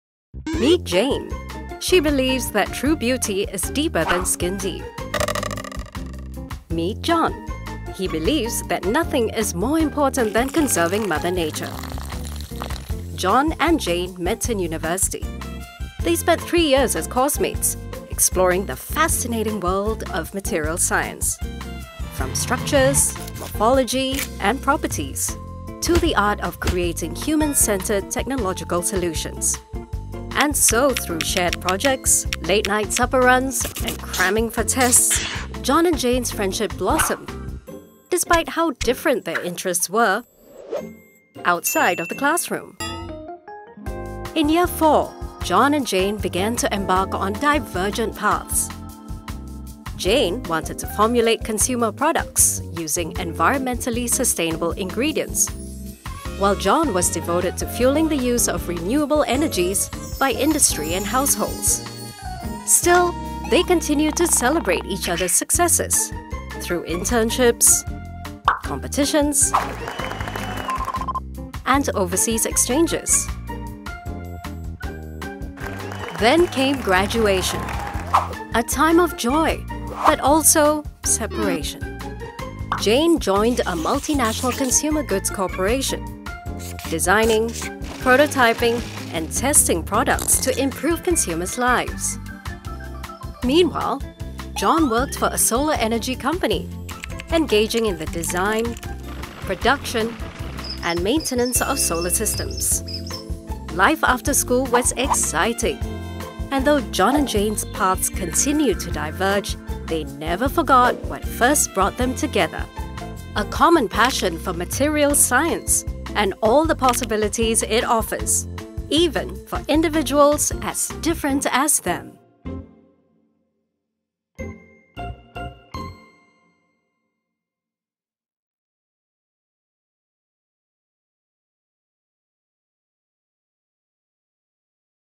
NTU School of Materials Science and Engineering - A Passion for Materials Science
Two Materials Engineering students with different interests share a common passion for materials science and upon graduation pursued their career paths in different industries.